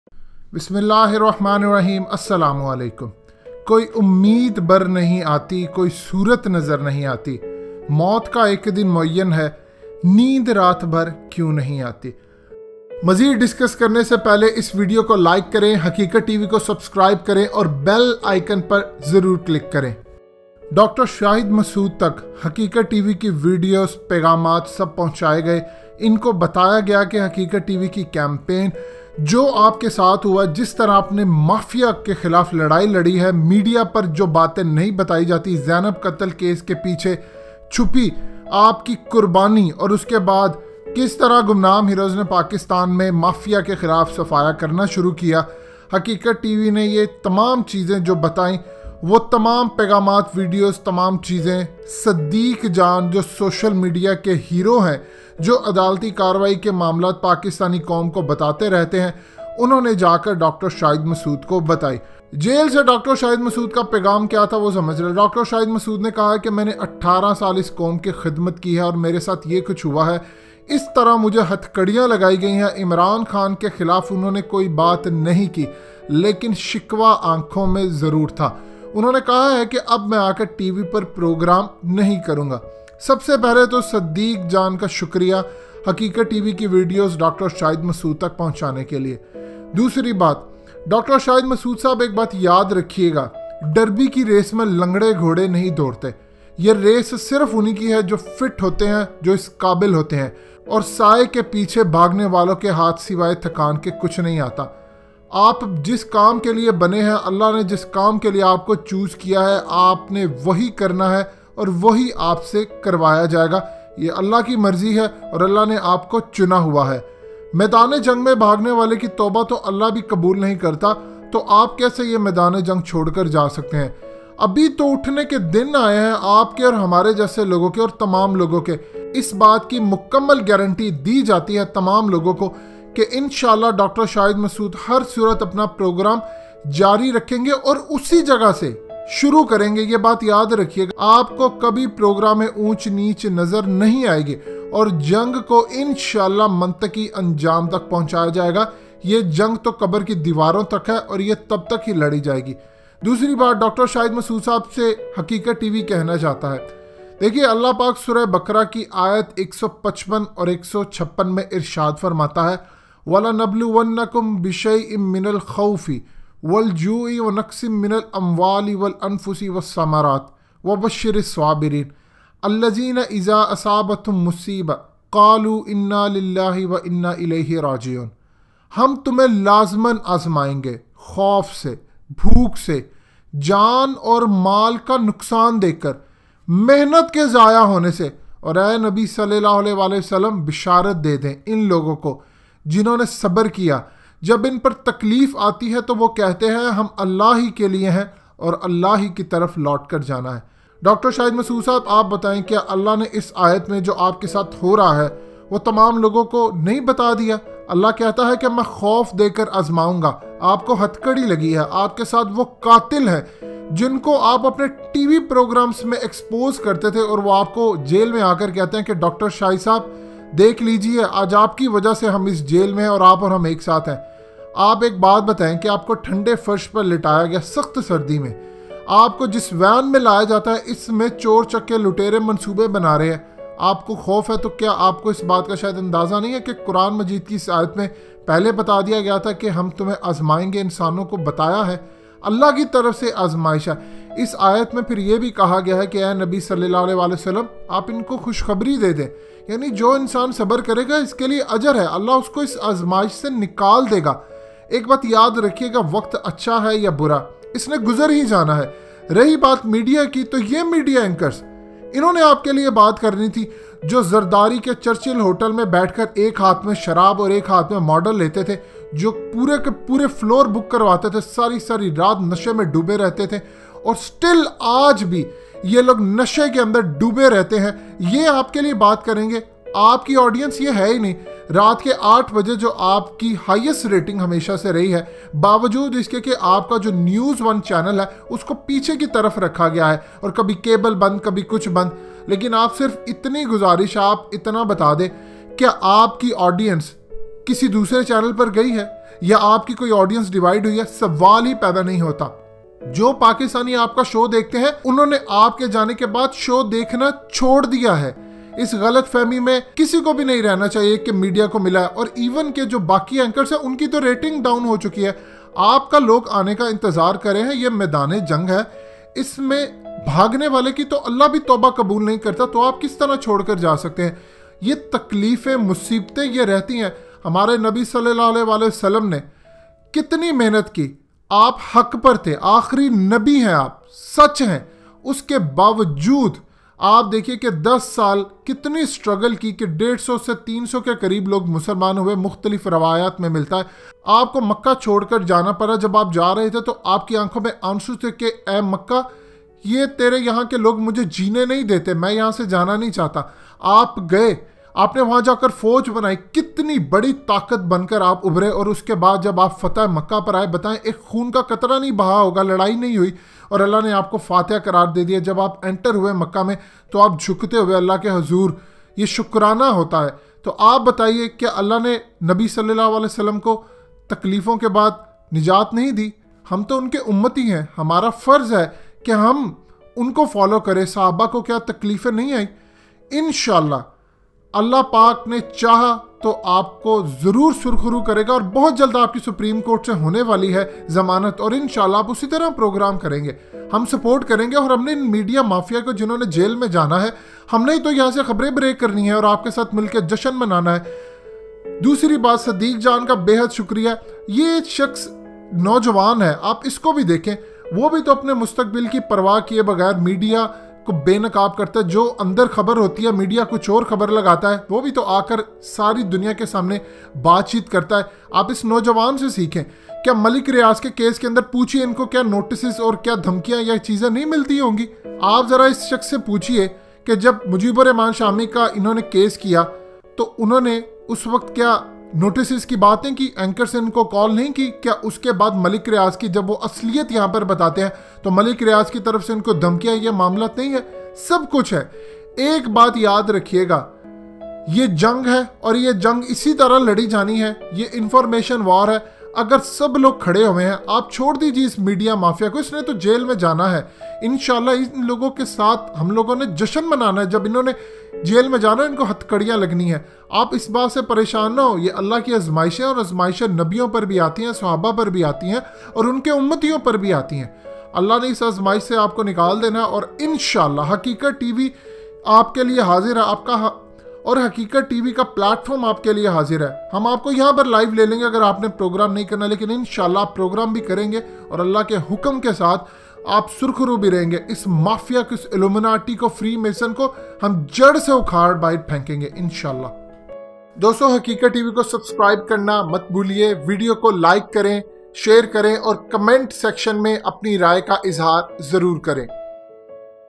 Siddique Jaan Gave Message of Haqeeqat Tv To Dr Shahid Masood
Dr Shahid Masood may come back and resume hi show but the famous guy Siddique Jaan gave him details about Haqeeqat TV and he talks about Imran Khan and PTI with Siddiquie.